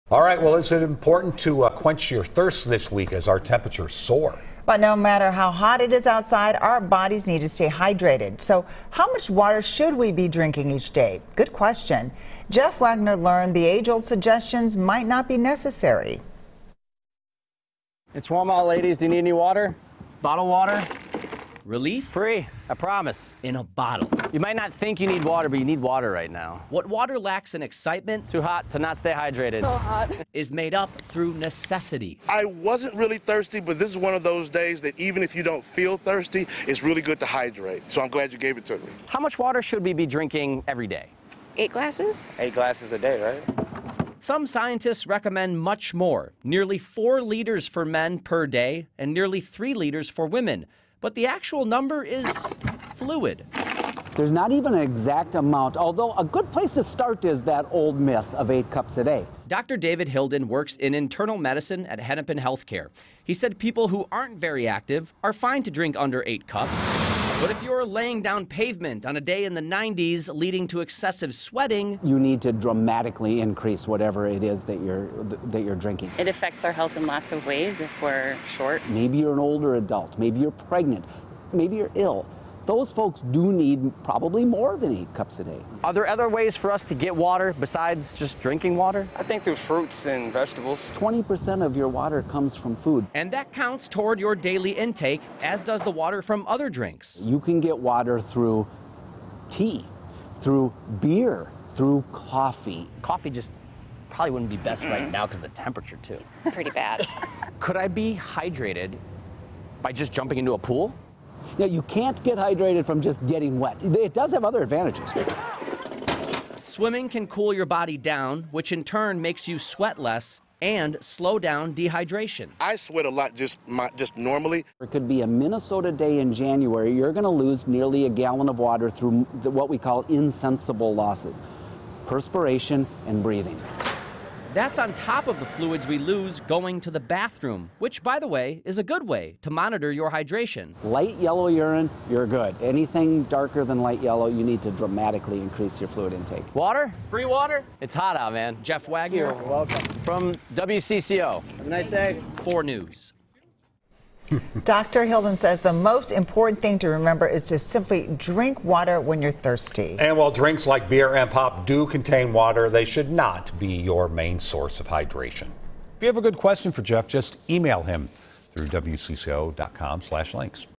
Good Question: How much water should we be drinking each day?
Jeff Wagner learned that the age-old suggestions might not be necessary.